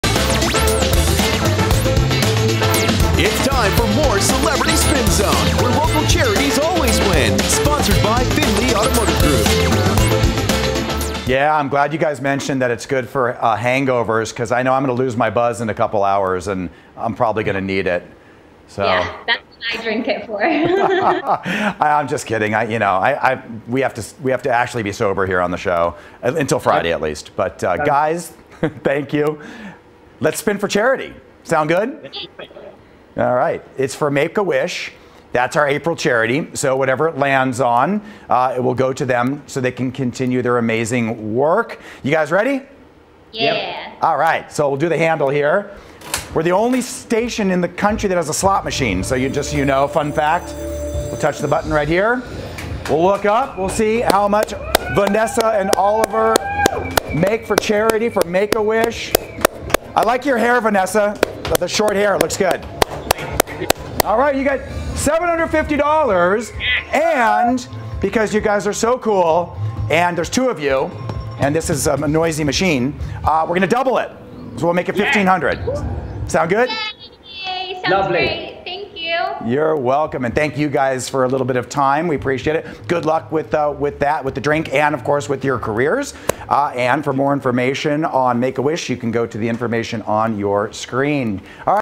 Vanessa Hudgens and Oliver Trevena spin to raise money Make-A-Wish Southern Nevada
Vanessa Hudgens and Oliver Trevena raises money for Make-A-Wish Southern Nevada in our Celebrity Spin Zone sponsored Findlay Auto Group.